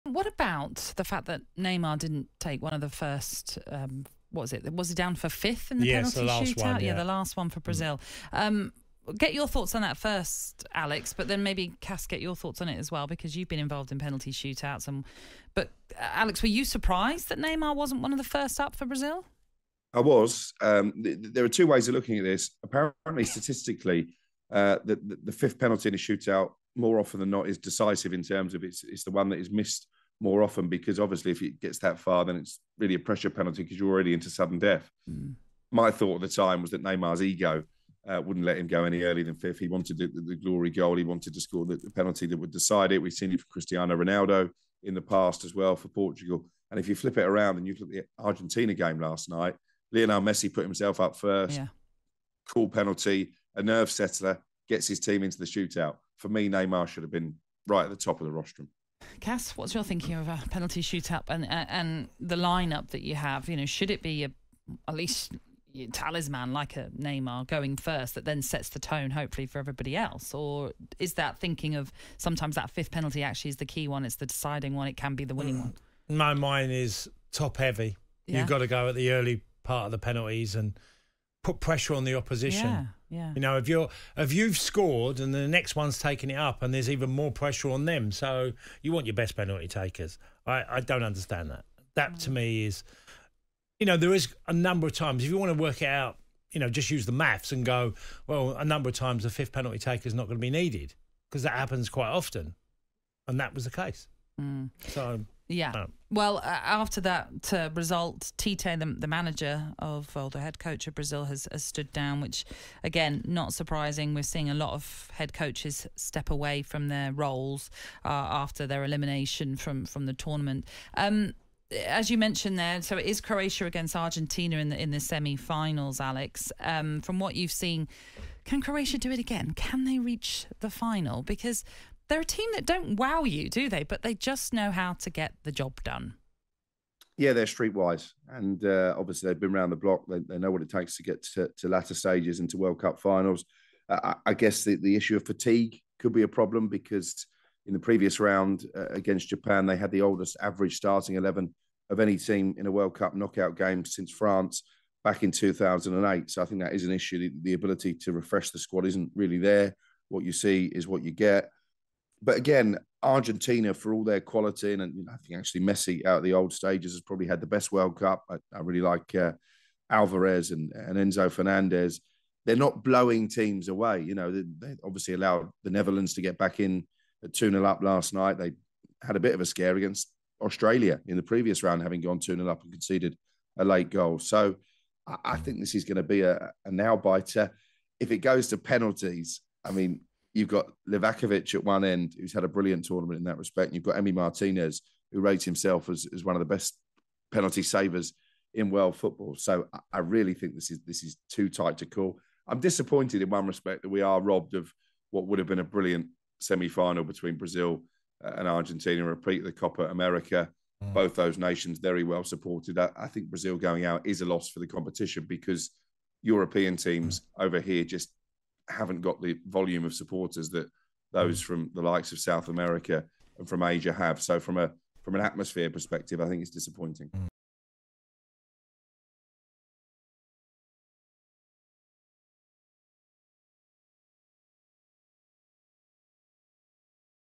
WHY DIDN'T NEYMAR TAKE A PENALTY?! 😡 Tony Cascarino can't believe Neymar didn't step up...
Tony Cascarino was left stunned by Neymar leaving himself as the fifth penalty taker vs Croatia in the quarter-finals!